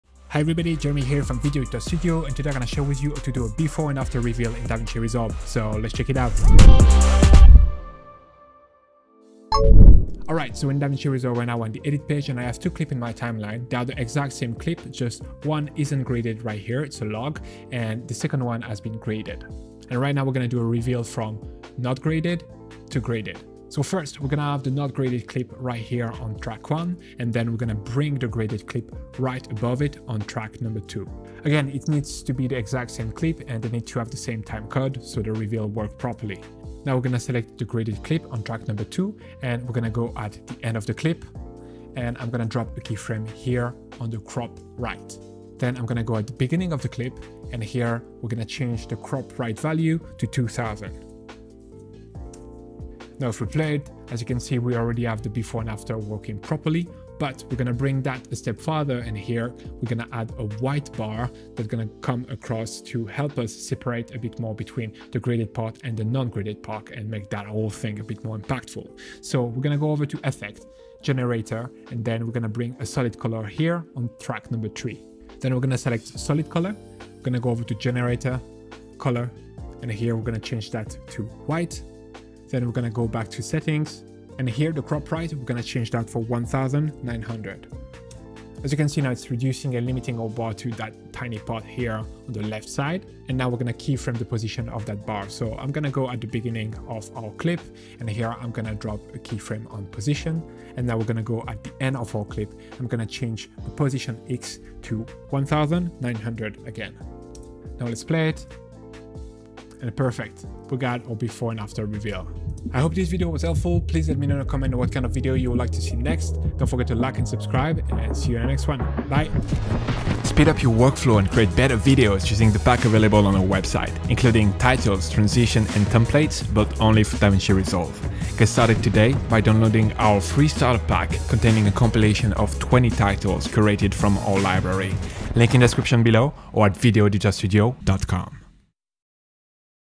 Before/After Wipe Slide Reveal Transition in Davinci Resolve
In this Davinci resolve tutorial I will share with you how to easily make a before and after reveal. It's very easy we can do everything from the edit page with simple key framing on the cropping. This effect is often used to reveal a color grading and emphasise the difference between the footage being graded and not graded.

I've provided timestamps so you can easily jump between sections:

00:00 - Intro
00:10 - How to reveal a color grade in Davinci Resolve?
00:45 - How to Keyframe the crop?
01:00 - How to add an animated white bar on the crop line?